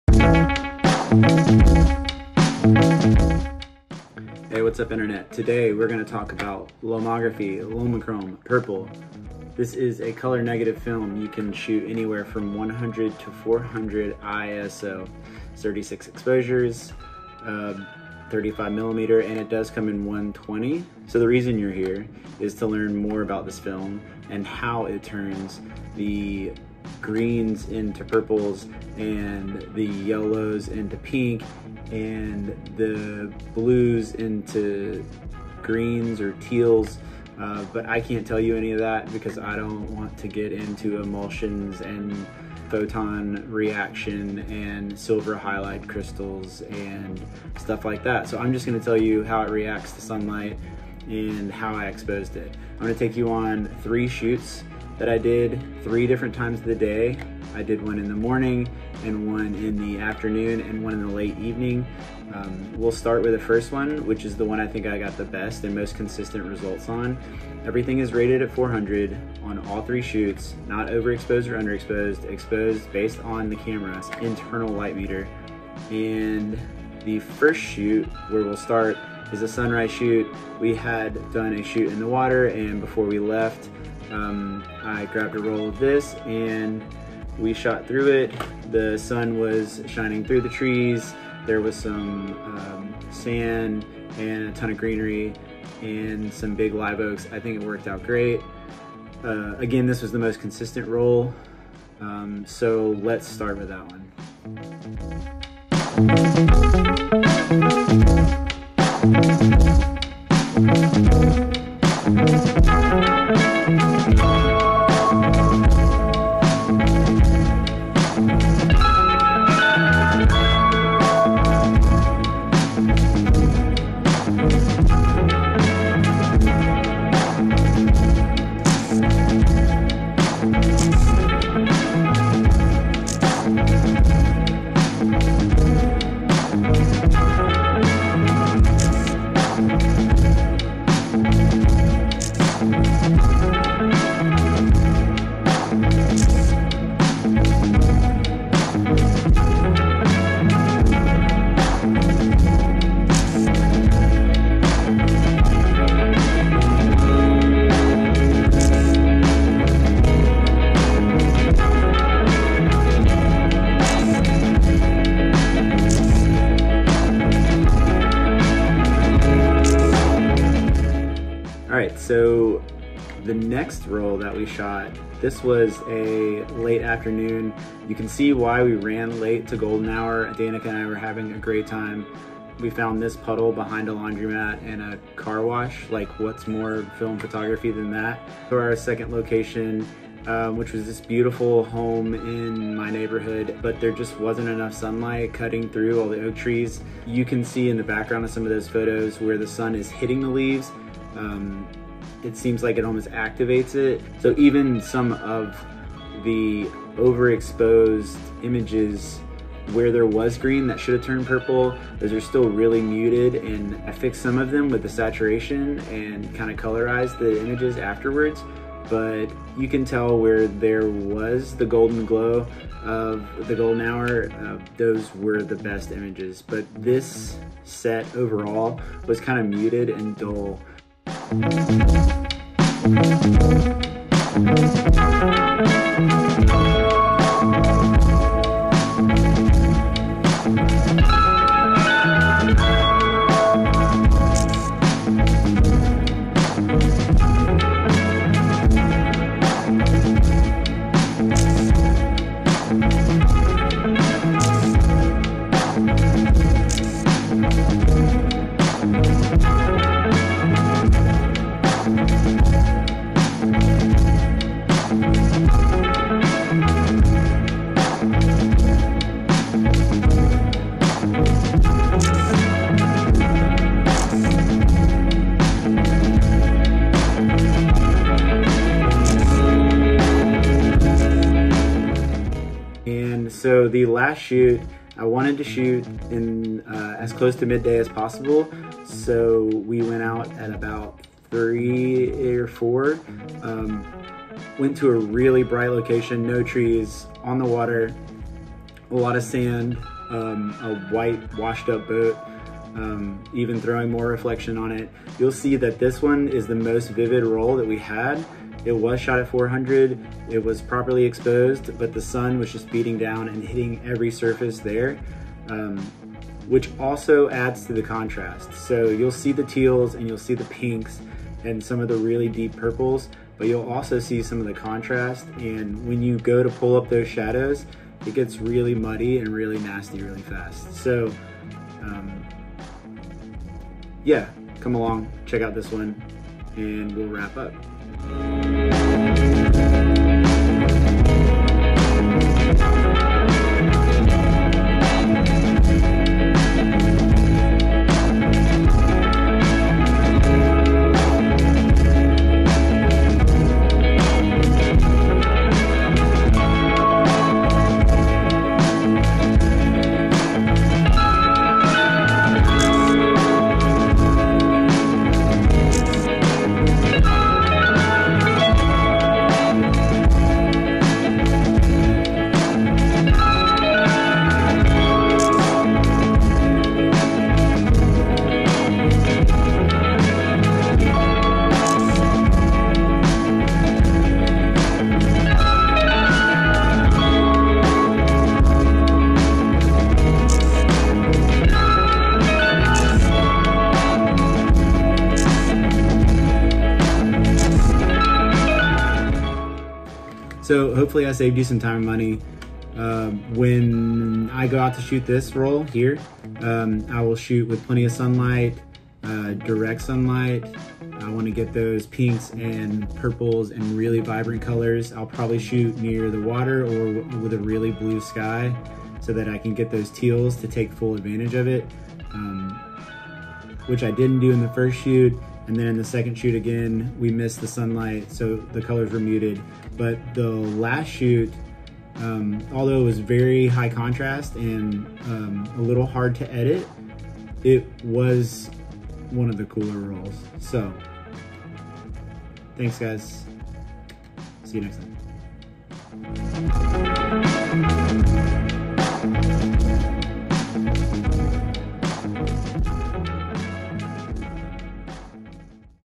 SHOOTING PORTRAITS ON LOMOGRAPHY LOMOCHROME PURPLE?!
In this video, I talk about Lomography Lomochrome Purple. I bring you along for three sessions to show you how the film reacts to different lighting throughout the day.

Camera: Nikon F3 / Zeiss 50mm F1.4 Planar or Nikon E-Series 28mm F2.8